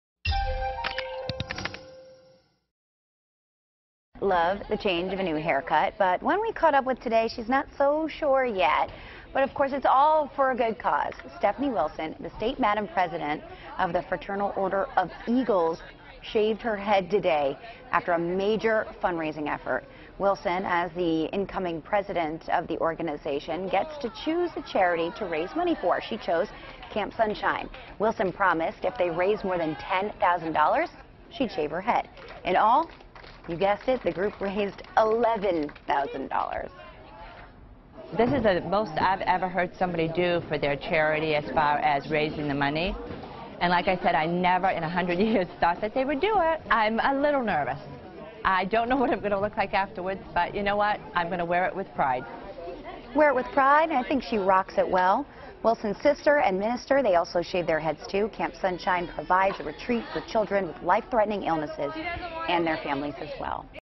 Eagles' Club president shaves head for charity
Three women are getting a new haircuts for a good cause.